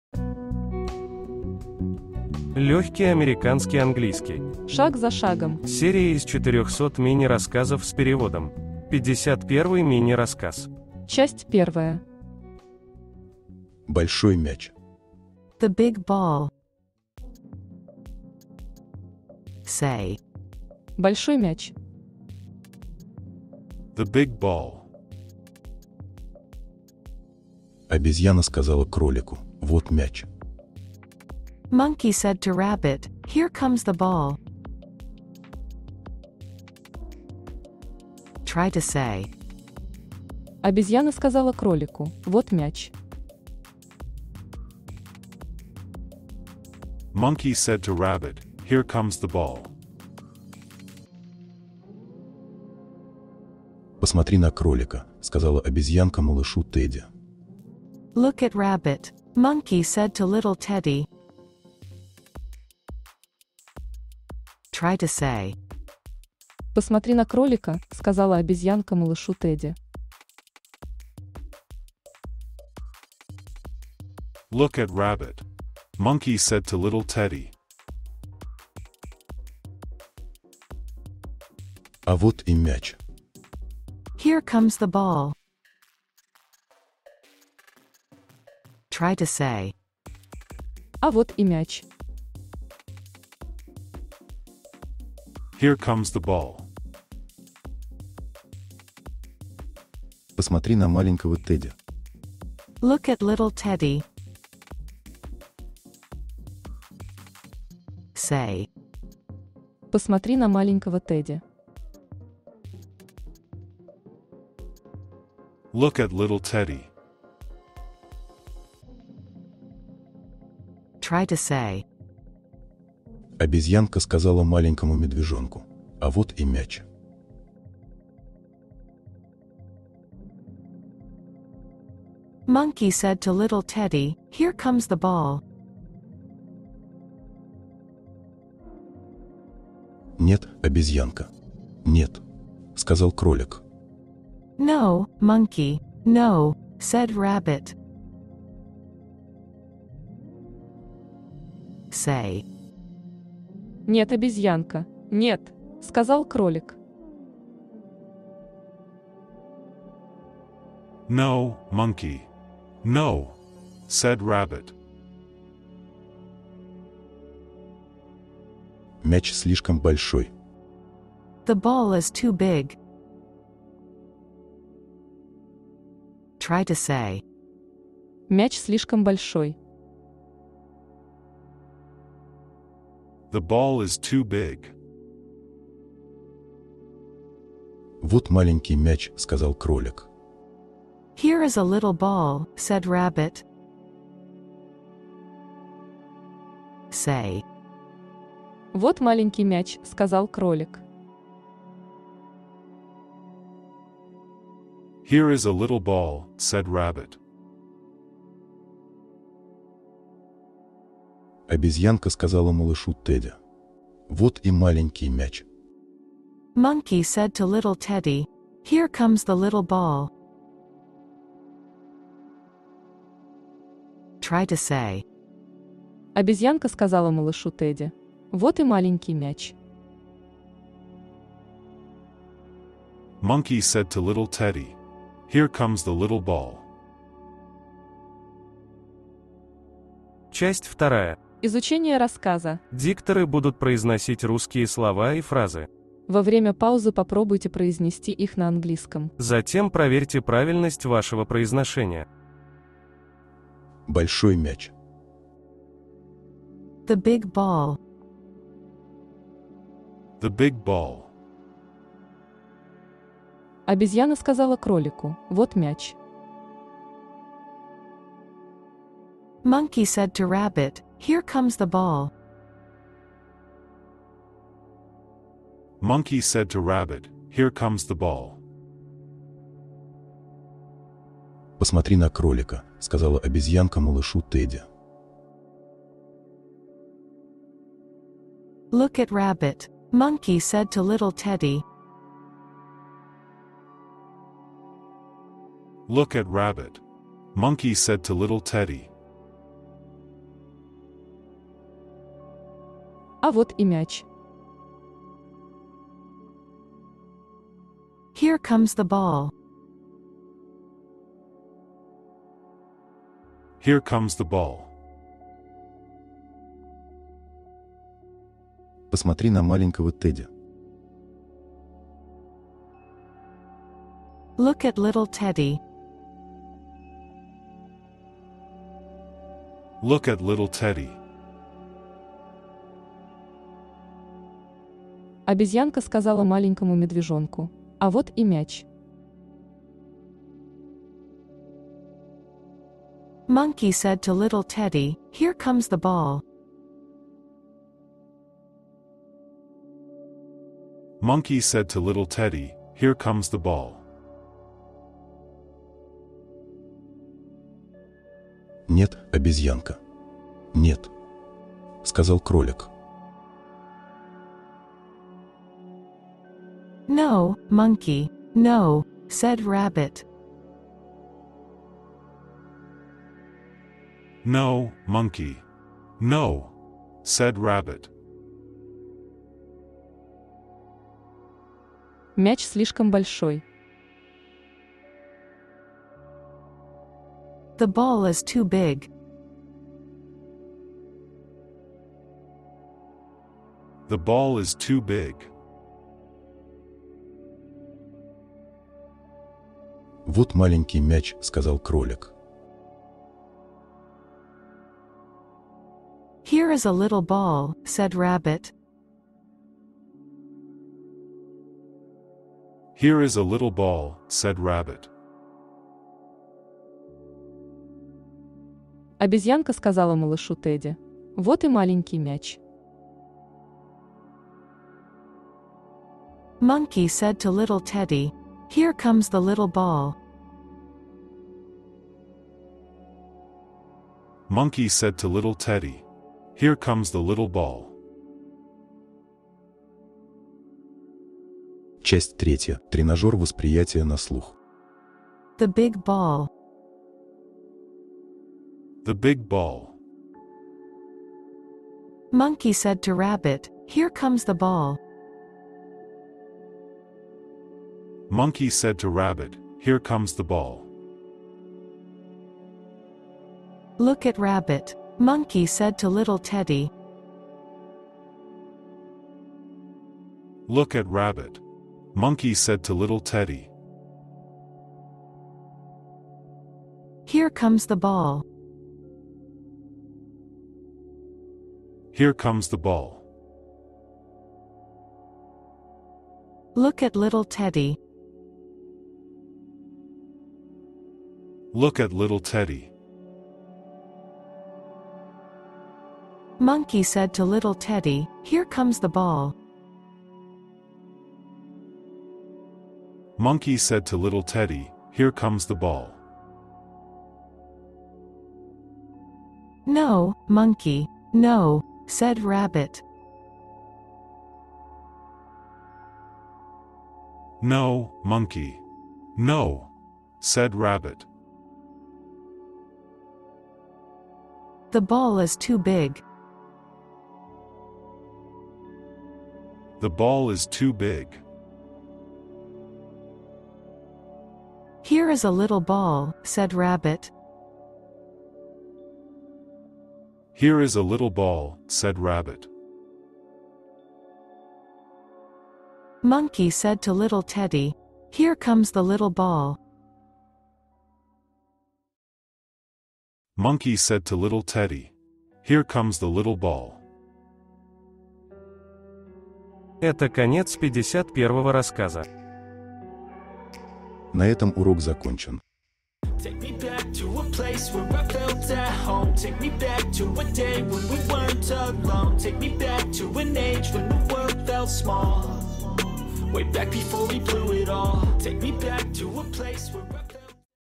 🎧51-й мини рассказ «The Big Ball» Американский английский третьего элементарного уровня
◘ Серия из четырехсот мини-рассказов с аудио-переводом. В помощь изучающим американский английский по методу доктора Пимслера.
00:00 Вступление.
00:13 Часть 1-я. Знакомство с текстом. Рассказ № 52 – «The Big Ball» - Большой Мяч.
04:13 Часть 2-я. Ваш перевод с русского на английский.
07:25 Часть 3-я. Тренажер восприятия на слух.